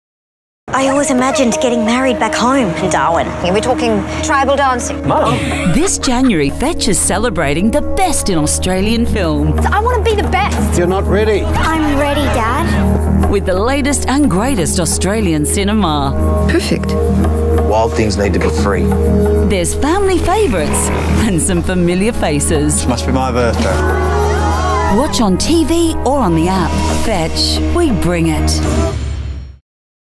Australian films on Fetch
Just in time for the Aussie long weekend – you’ll find the best in Australian film in the Movie Store on Fetch!